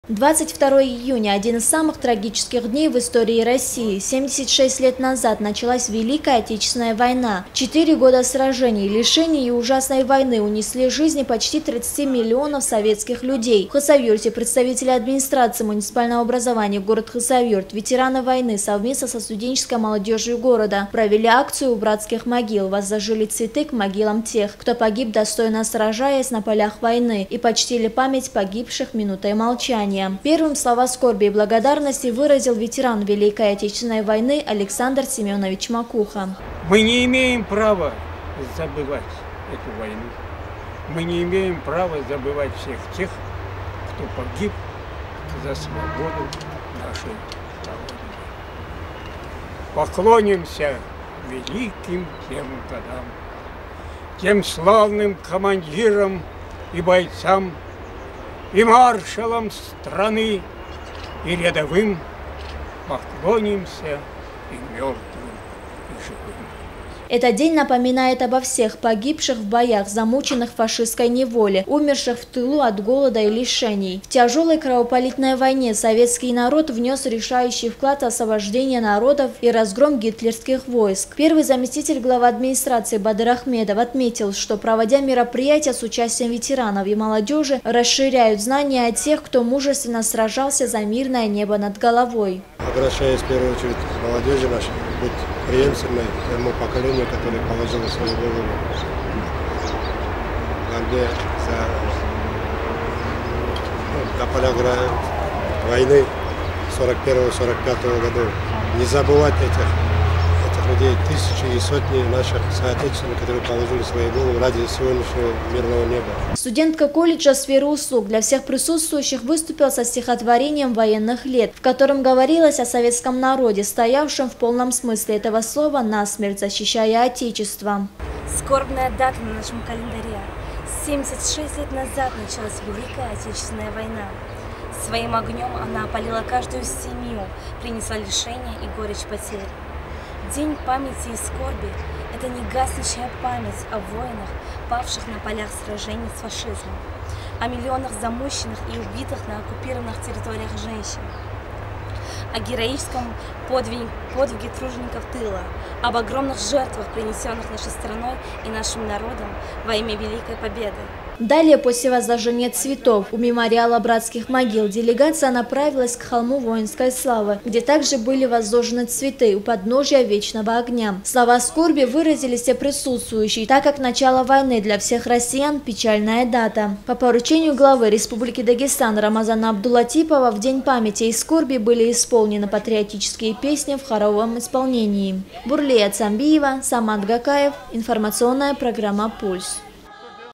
День памяти и скорби в Хасавюрте
День начала Великой Отечественной войны, 22 июня, в России объявлен днем памяти и скорби. Приурочив, к данной дате работники администрации совместно со студенческой молодежью города посетили братские могилы, где похоронены самые отважные и смелые труженики военных лет, погибшие на полях сражений. После делегация отправилась к холму воинской славы, где была собрана вся общественность города, почтить память погибших.  Все подробности в следующем материале.